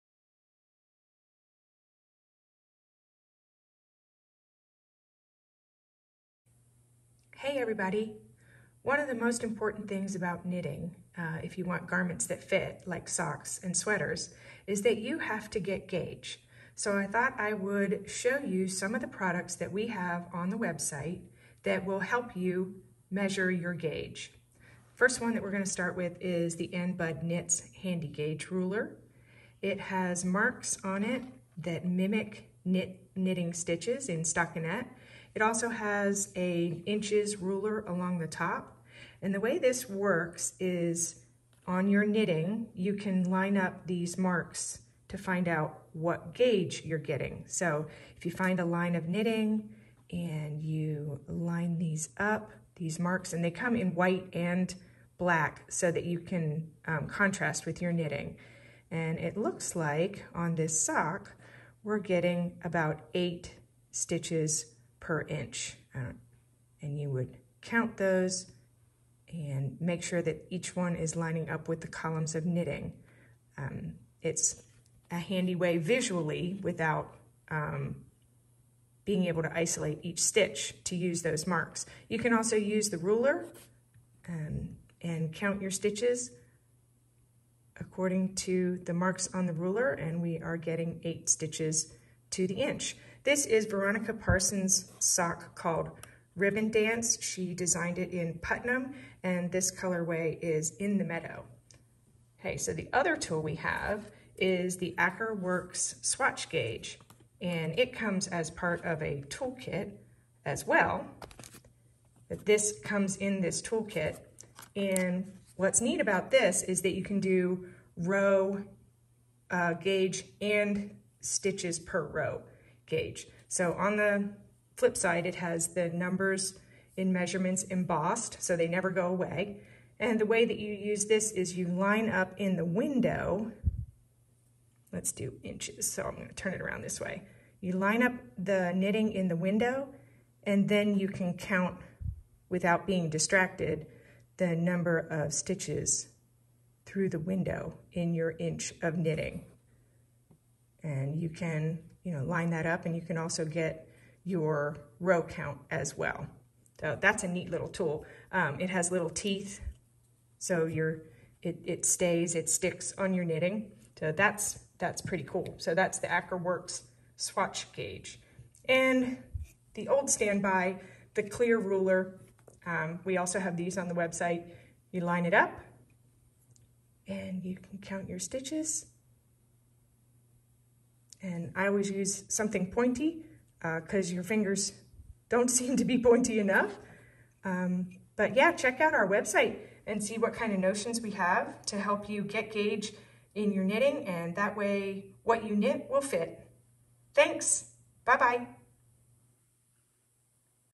Gauge Tools
Jennifer demonstrates a selection of tools used for measuring your gauge (stitches and rows per inch) in knitting.

00:00 - Intro
00:27 - Ann Budd Handy Gauge Ruler
01:36 - Veronica Parsons' sock pattern, Ribbon Dance
01:46 -Aker Works Swatch Gauge
02:52 - Miss Babs clear ruler